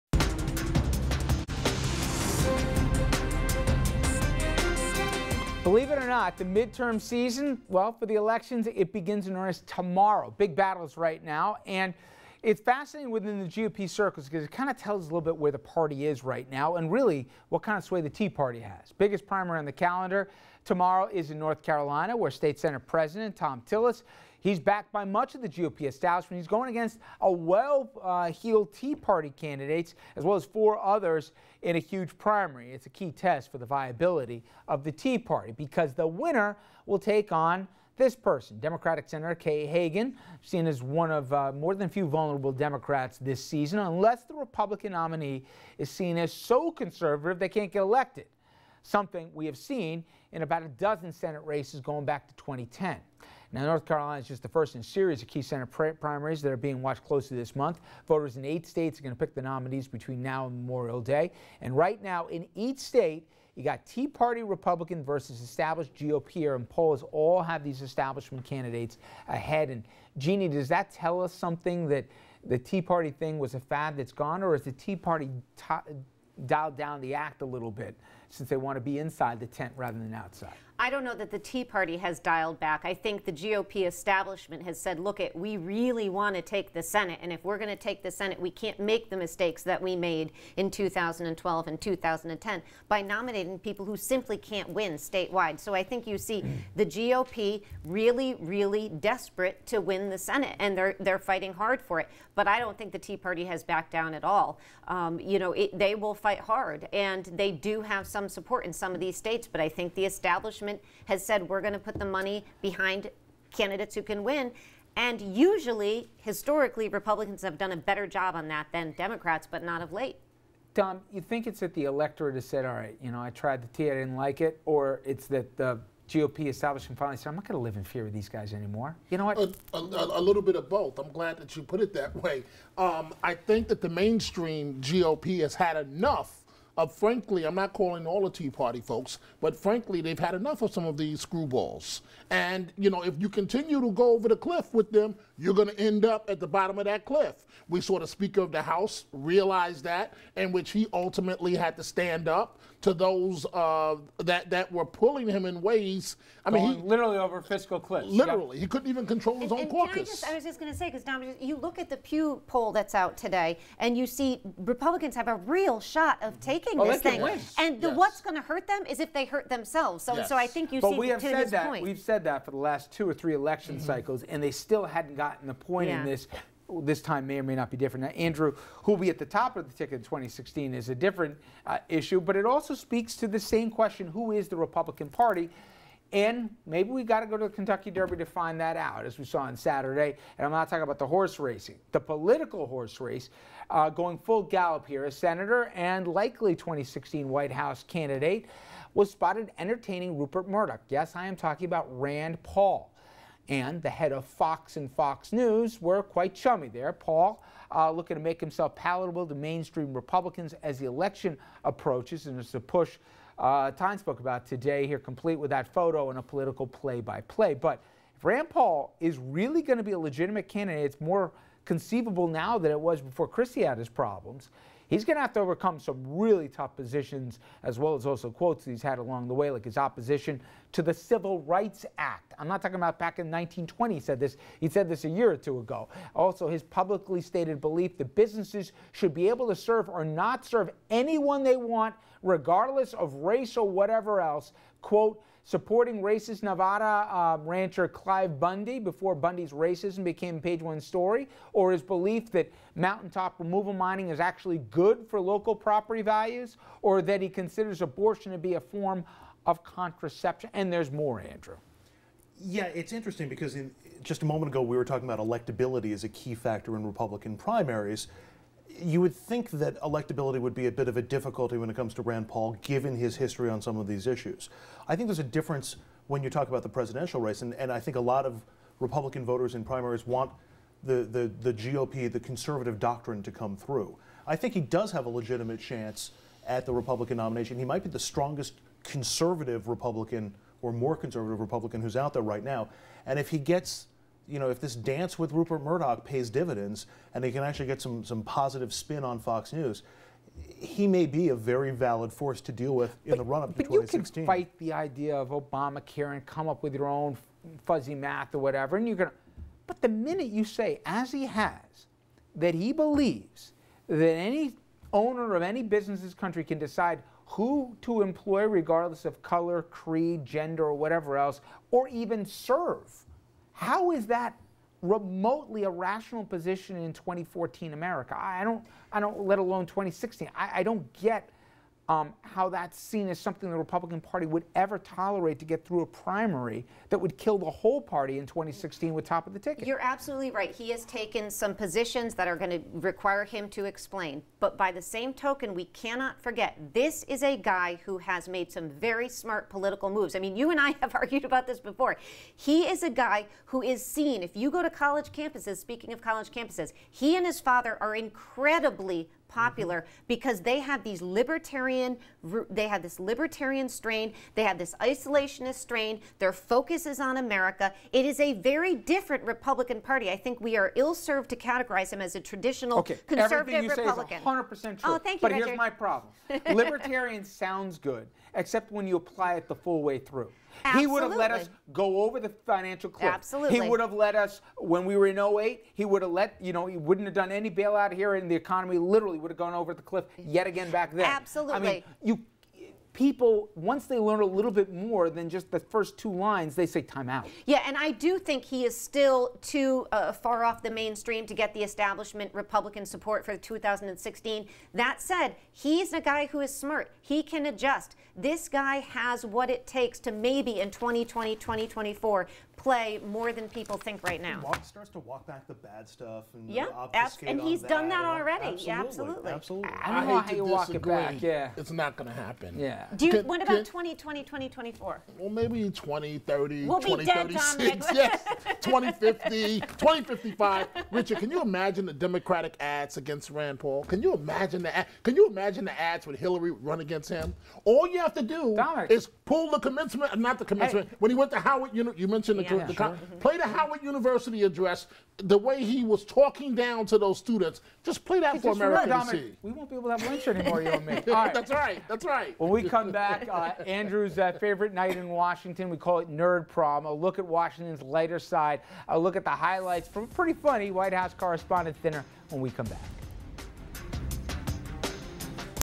America Votes 2014 - Tea Party VS. GOP In Primaries Tomorrow
Richard French speaks to the Panel about Rand Paul's possible GOP nomination in 2016.